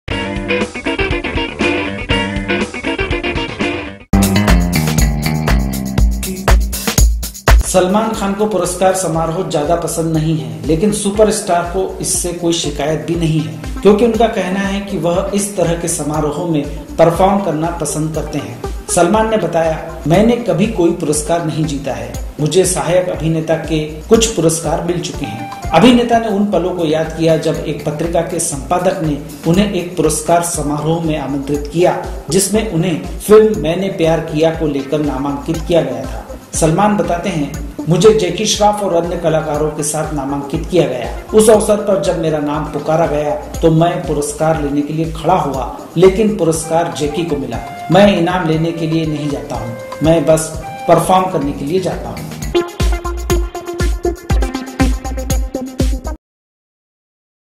मैं पुरस्कार के लिए खड़ा हुआ और वो जैकी को मिल गया : सलमान
सलमान खान को पुरस्कार समारोह ज्यादा पसंद नहीं है, लेकिन सुपरस्टार को इससे कोई शिकायत भी नहीं है क्योंकि उनका कहना है कि वह इस तरह के समारोहों में परफार्म करना पसंद करते हैं। सलमान ने बताया ‘‘मैंने कभी कोई पुरस्कार नहीं जीता है। मुझे सहायक अभिनेता के कुछ पुरस्कार मिल चुके हैं।’’